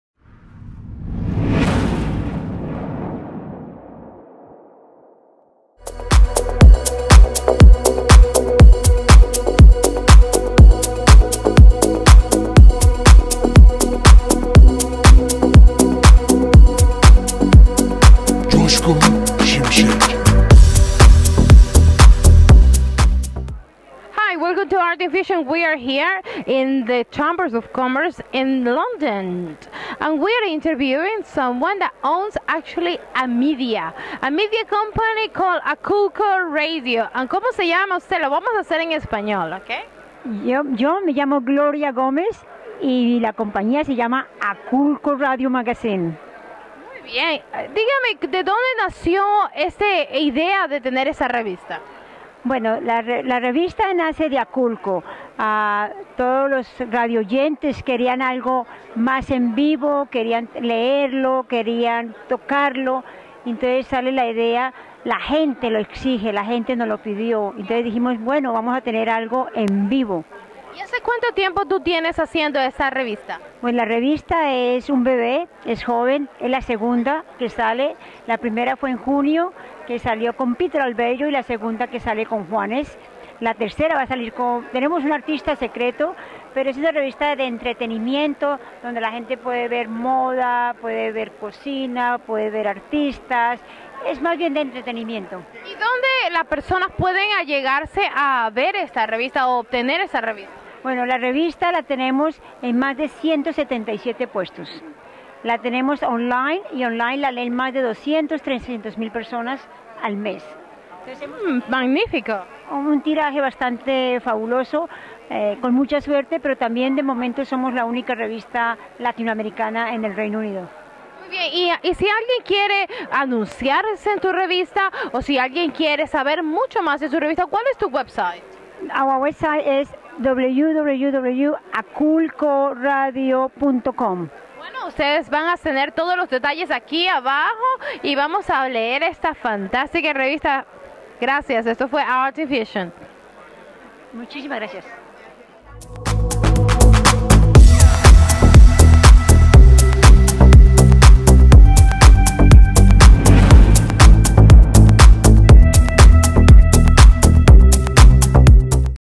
ART IN FUSION TV - Entrevista con Gloria Gomez - Aculco Magazine
Entrevista con Gloria Gomez Durante la Feria de Servicios & Negocios en Londres: Organizada  por la Camara de Comercio de Colombia